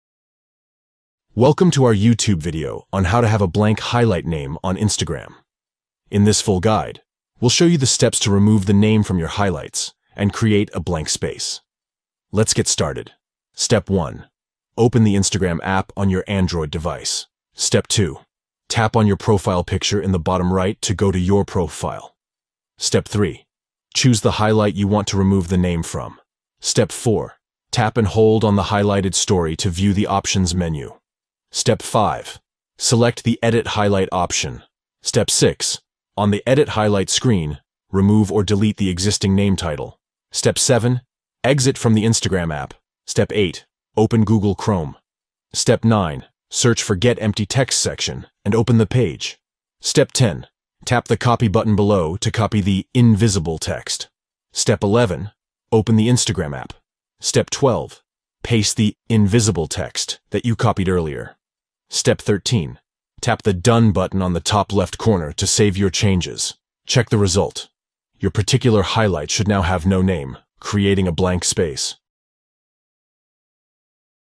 How To Have Blank Highlight Name On Instagram - Full Guide
In this video, we'll show you how to have a blank highlight name on Instagram in just a few easy steps:

1. Open Instagram app on your Android device.
2. Tap on your profile picture in the bottom right to go to your profile.
3. Choose the highlight you want to remove the name from.
4. Tap and hold on the highlighted story to view the options menu.
5. Choose ‘Edit highlight’ option.
6. On the ‘Edit Highlight’ screen, remove or delete existing name/title.
7. Exit from Instagram app.
8. Open Google Chrome.
9. Search for "Get empty text section", and open the page.
10. Tap the ‘Copy’ button below to copy the “invisible text”.
11. Open the Instagram app.
12. Paste the “invisible text” that you copied earlier here.
13. Tap the ‘Done’ button on the top left corner, to save your changes.

Check the result. Your highlight should now have no name.